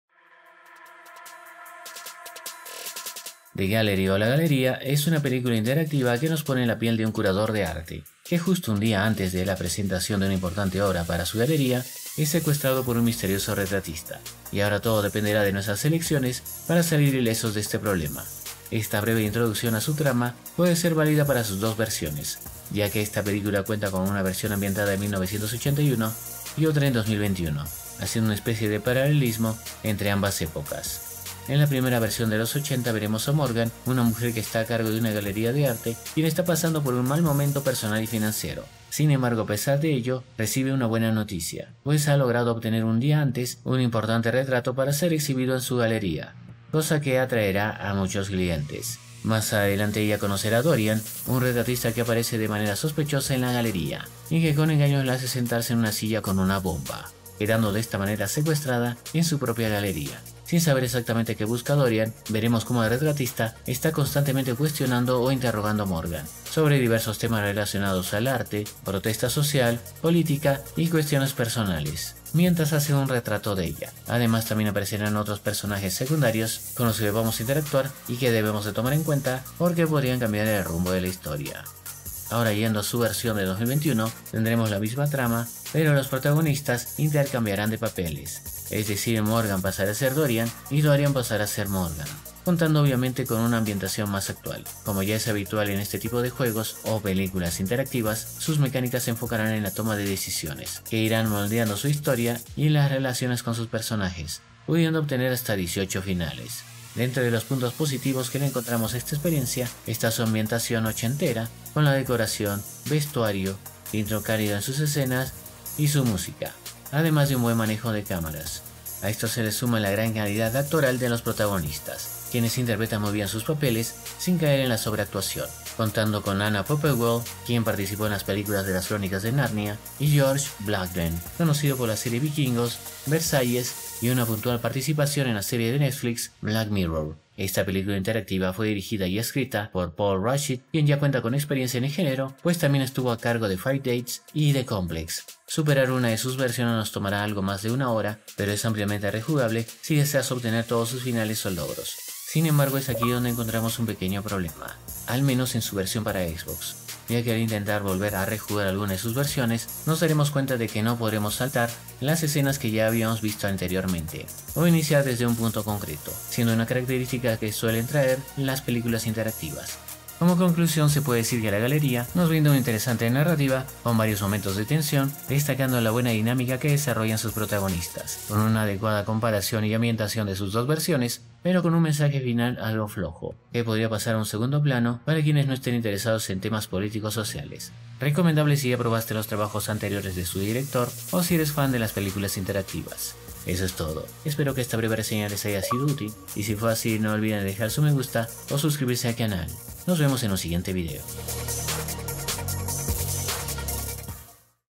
🎨The GALLERY / La GALERÍA - FMV | Reseña en Español
Mini reseña de The Gallery o La Galería.

▇▇▇▇▇▇▇ DATOS DEL JUEGO ▇▇▇▇▇▇▇
Desarrollado y Publicado por:  Aviary Studios
Lanzamiento en Xbox: 15 de septiembre del 2022
Disponible en: Playstation, Xbox, Nintendo Swith y PC
Esta reseña fue posible gracias una copia digital del juego enviada por su desarrollador/editor.